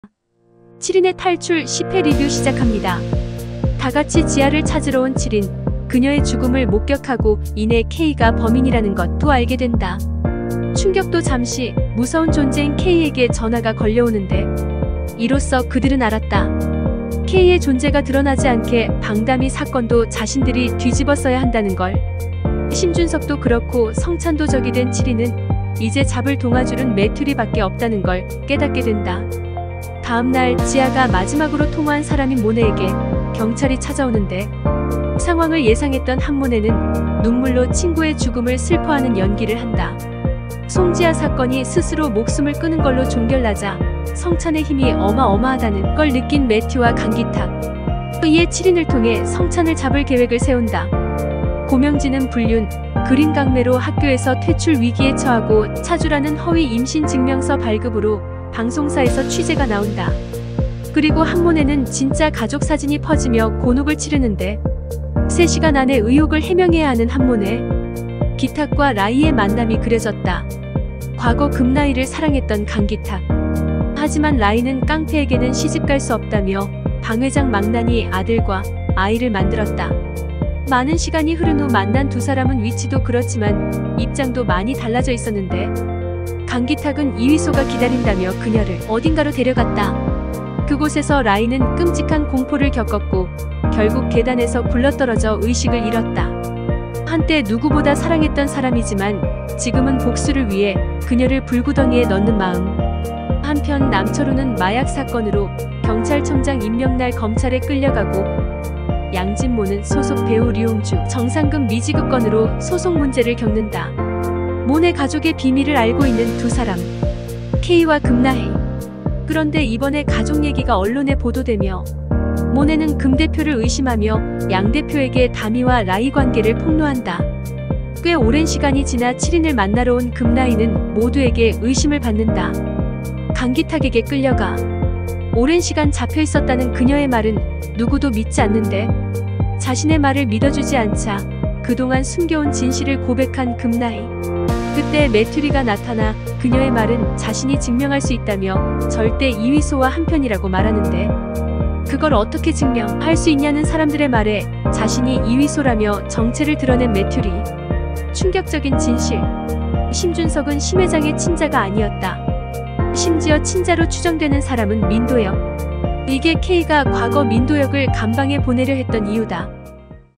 7인의 탈출10회, 민도혁이 성찬그룹 진짜 아들?
7인의 탈출 10회 리뷰 시작합니다

다같이 지아를 찾으러 온 7인, 그녀의 죽음을 목격하고 이내 K가 범인이라는 것도 알게 된다. 충격도 잠시 무서운 존재인 K에게 전화가 걸려오는데...

이로써 그들은 알았다. K의 존재가 드러나지 않게 방다미 사건도 자신들이 뒤집어 었어야 한다는 걸.. 심준석도 그렇고 성찬도 적이 된 7인은 이제 잡을 동아줄은 매튜리 밖에 없다는 걸 깨닫게 된다.

다음날, 지아가 마지막으로 통화한 사람인 모네에게 경찰이 찾아오는데..

상황을 예상했던 한모네는 눈물로 친구의 죽음을 슬퍼하는 연기를 한다.

송지아 사건이 스스로 목숨을 끊은 걸로 종결 나자 성찬의 힘이 어마 어마하다는 걸  느낀 매튜와 강기탁. 이에 7인을 통해 성찬을 잡을 계획을 세운다.

고명지는 불륜, 그림 강매로 학교에서 퇴출 위기에 처하고 차주란은 허위 임신 증명서 발급으로 방송사에 서 취재가 나온다. 그리고 한모네는 진짜 가족사진이 퍼지며 곤혹을 치르는데..
3 시간 안에 의혹을 해명해야 하는 한모네!
기탁과 라희의 만남이 그려졌다. 과거 금라희를 사랑했던 강기탁. 하지만 라희는 깡패에게는 시집갈 수 없다며 방회장 망나니 아들과 아이를 만들었다. 많은 시간이 흐른 후 만난 두 사람은 위치도 그렇지만 입장도 많이 달라져 있었는데... 강기탁은 이휘소가 기다린다며 그녀를 어딘가로 데려갔다.
그곳에서 라희는 끔찍한 공포를 겪었고 결국 계단에서 굴러떨어져 의식을 잃었다. 이걸로 보아 기탁 라희 사이에 가장 많이 변한 건 기탁의 마음임을 알 수 있었다. 한때 누구보다 사랑했던 사람이지만 지금은 복수를 위해 그녀를 불구덩이에 넣는 마음.

한편, 남철우는 마약사건으로 경찰청장 임명 날 검찰에 끌려가고 양진모는 소속 배우 류홍주로부터 정산금 미지급 건으로 소송 문제를 겪는다.

가족의 비밀을 알고 있는 두 사람. K와 금라희. 그런데 이번에 가족 얘기가 언론에 보도되며
모네는 금대표를 의심하며 양대표에게 다미와 라희 관계를 폭로한다.

꽤 오랜 시간이 지나 7인을 만나러 온 금라희는 모두에게 의심을 받는다. 강기탁에게 끌려가
오랜 시간 잡혀 있었다는 그녀의 말은 누구도 믿지 않는데..
자신의 말을 믿어주지 않자 그동안 숨겨온 진실을 고백한 금라희! 그때, 매튜리가 나타나 그녀의 말은 자신이 증명할 수 있다며 절대 이휘소와 한편이라고 말하는데... 그걸 어떻게 증명할 수 있냐는 사람들의 말에 자신이 이휘소라며 정체를 드러낸 매튜리!
충격적인 진실! 심준석은 심회장의 친자가 아니였다. 심지어 친자로 추정되는 사람은 민도혁!

이게 K가 과거 민도혁을 감방에 보내려 했던 이유다.